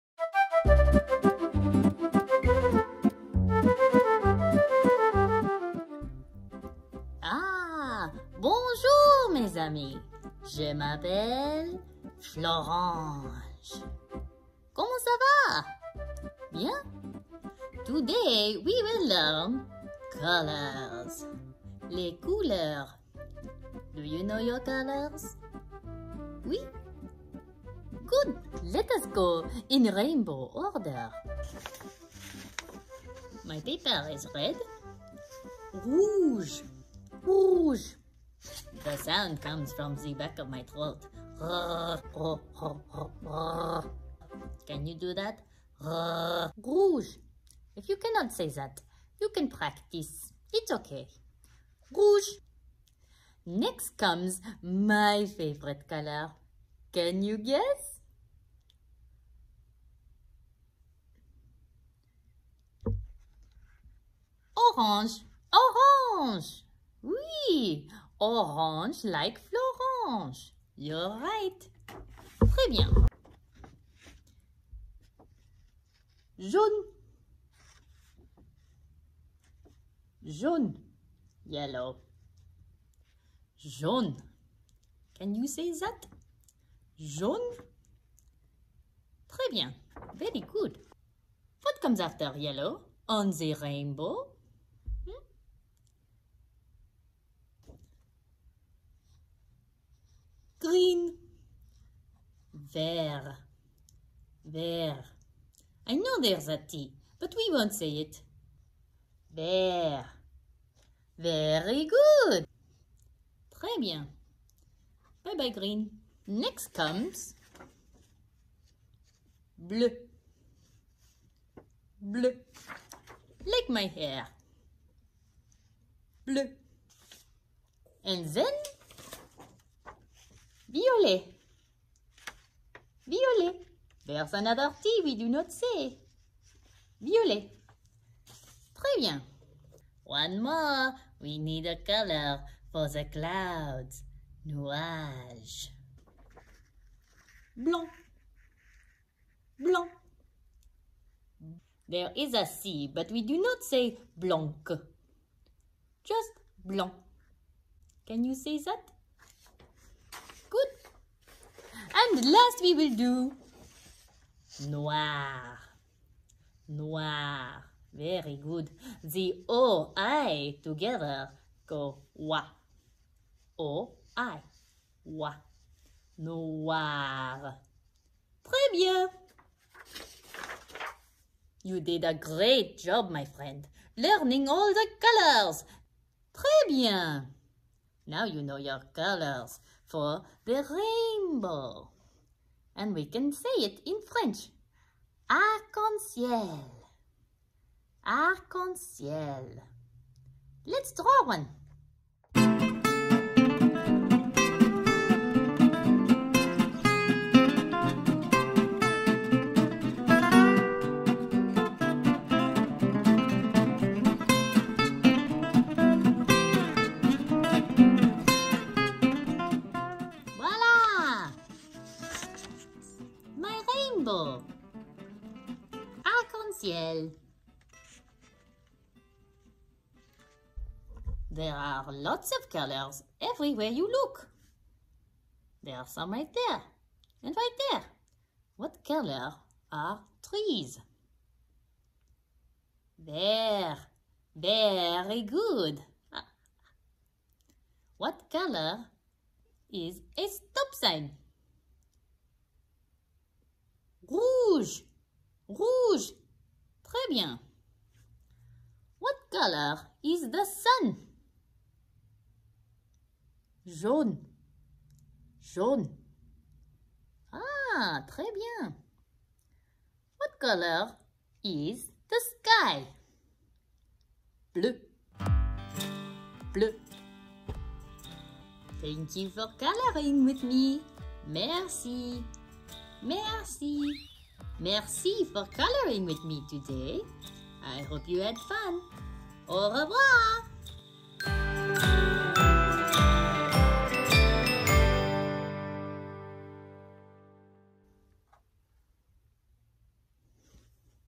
French with Florenge | Colors
Hi Buddies!  Learn the colors in French with our favorite furry friend, Florenge!  Be sure to repeat the words after Florenge.  Focus on copying the sound.